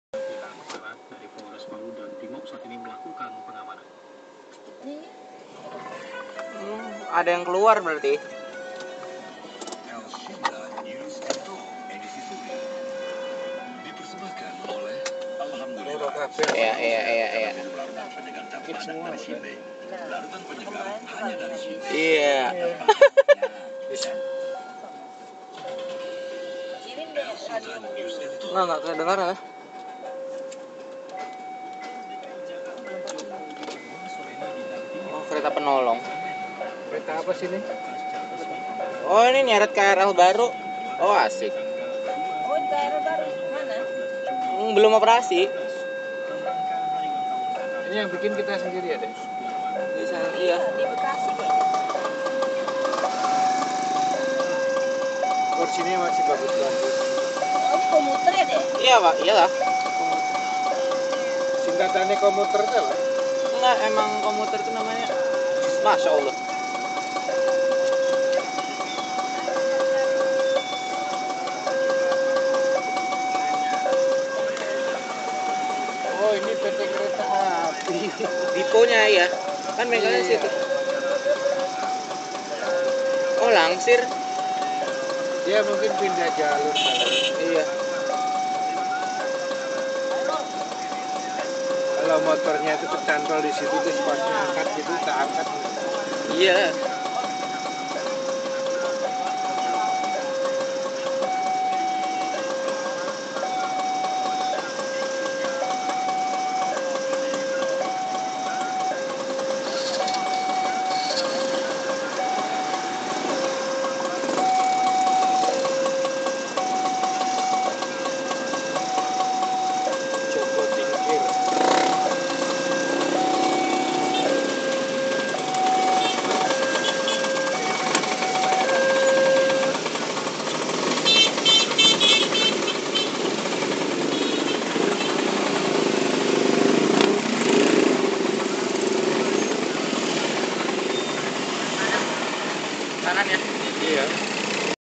Perlintasan Kereta Api Dipo Bukit Duri
KRD Penolong Joko Tingkir menarik KRL KfW untuk dilangsir di Dipo KRL Bukit Duri

Diambil sore tadi jam 16:00